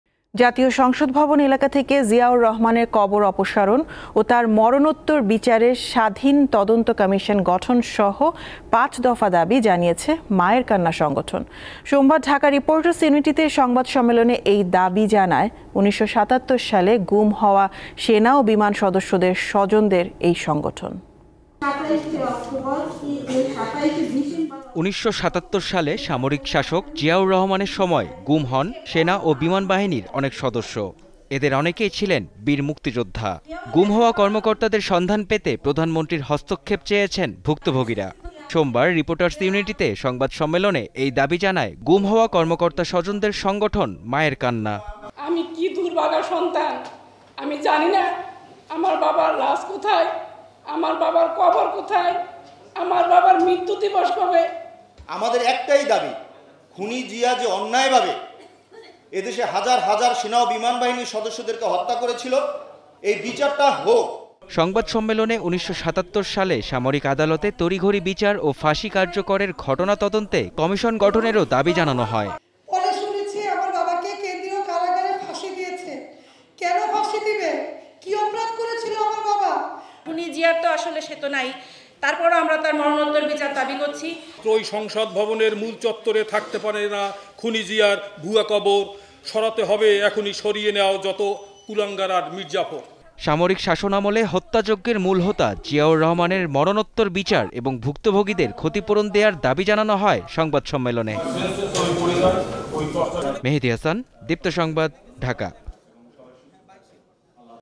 সংসদ থেকে জিয়াউর রহমানের সমাধি অপসারণ চায় মায়ের কান্না সংগঠন | Ziaur Rahman | Deepto News
Address:
Deepto TV
7/A/GA Tejgaon Industrial Area,